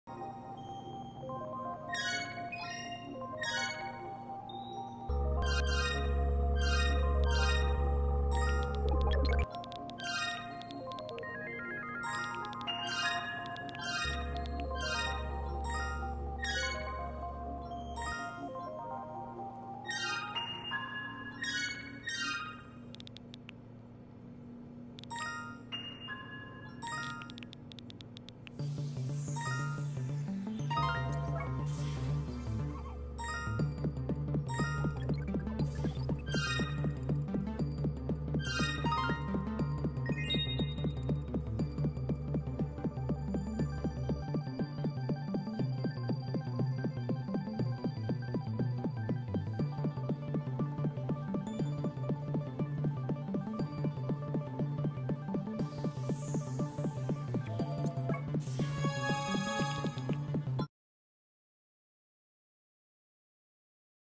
What I got from trading the MERMAID HALO!(was it fair?)!
This is what I got from trading the mermaid halo!